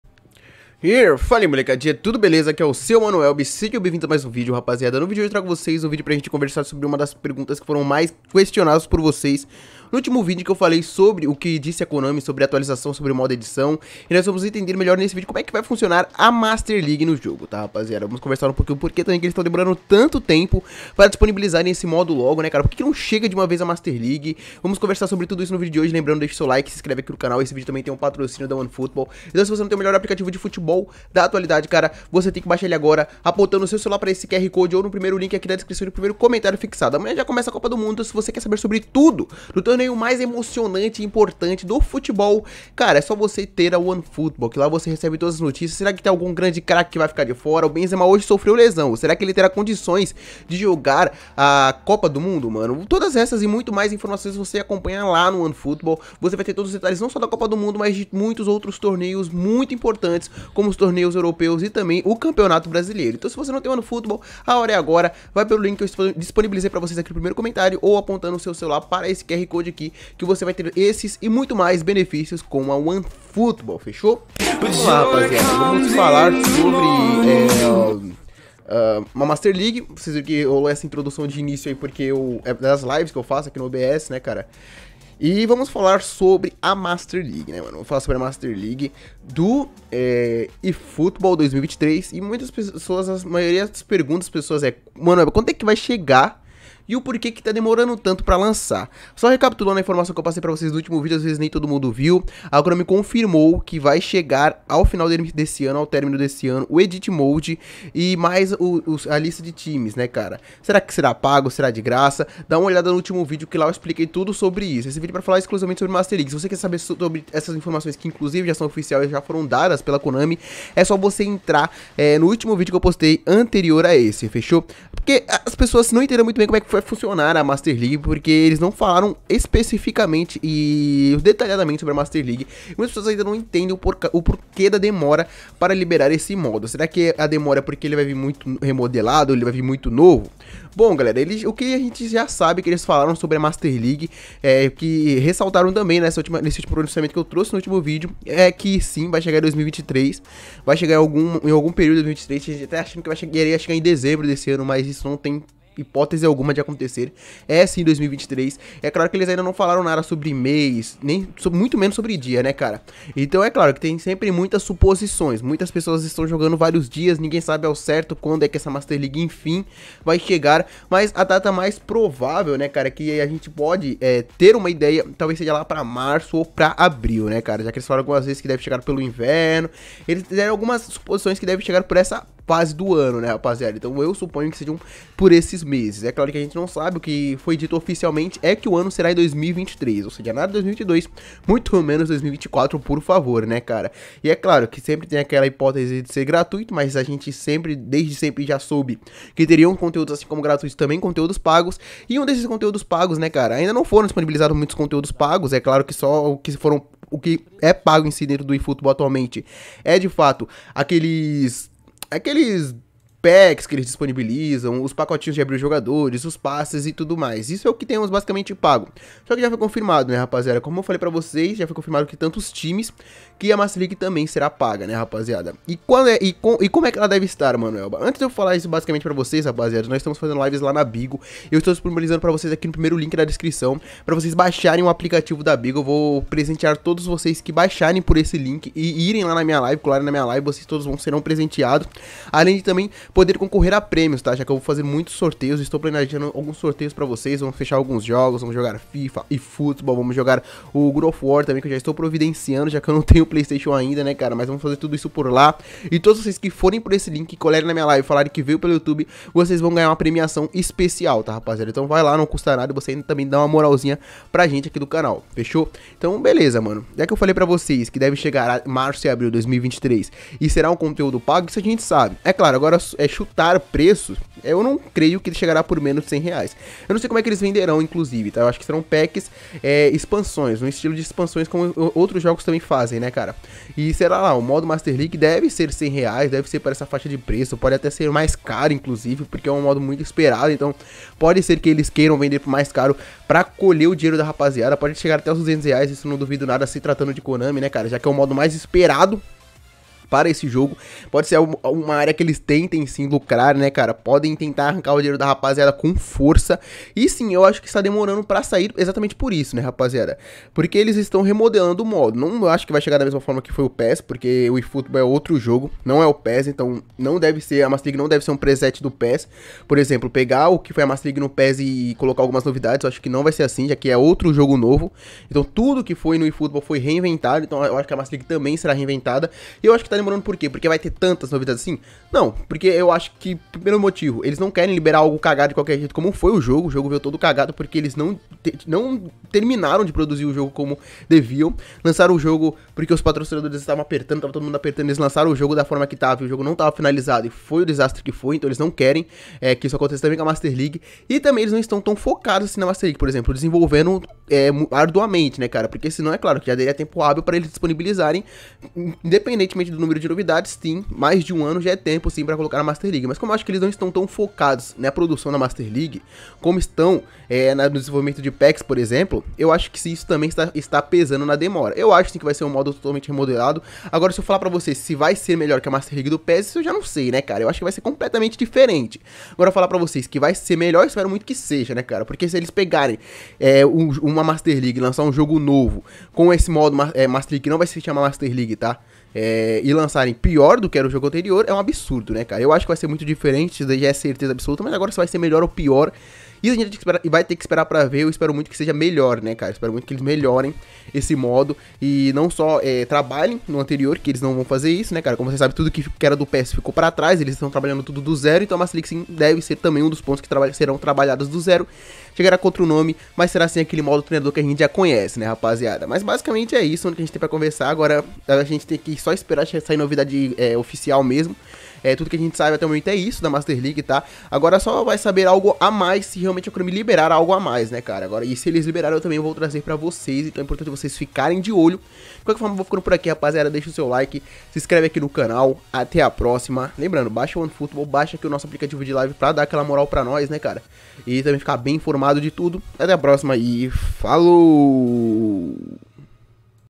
KONAMI FALOU OFICIALMENTE SOBRE A MASTER LEAGUE NO EFOOTBALL 2023!
ManooElba100k

Parceiros: W2I Games, Onefootball